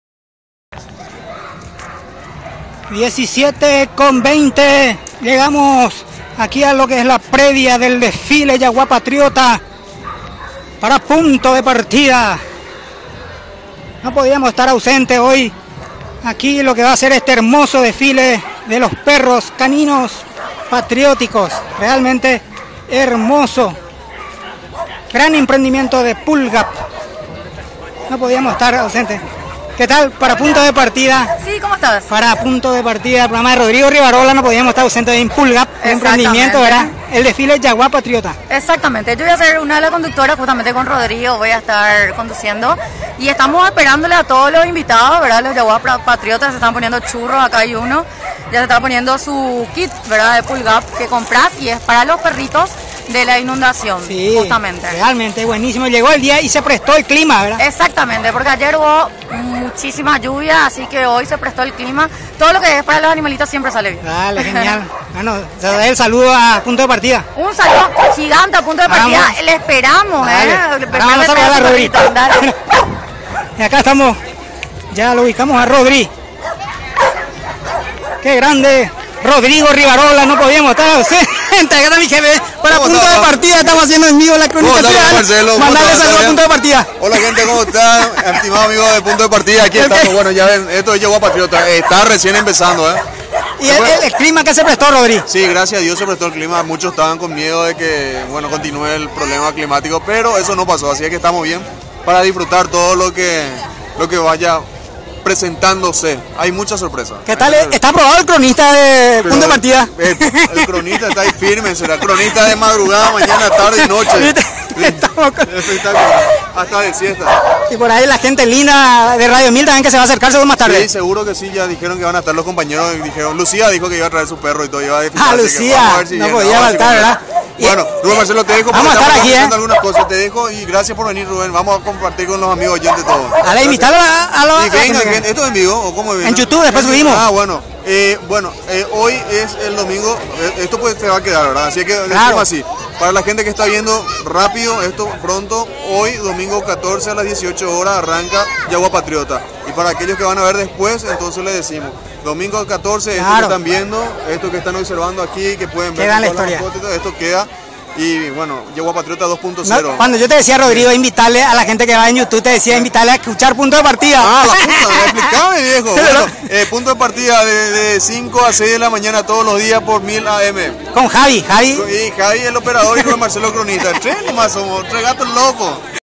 DESFILE JAGUA PATRIOTA !!
Pulgapp y el hermoso Desfile de Caninos frente a la Catedral.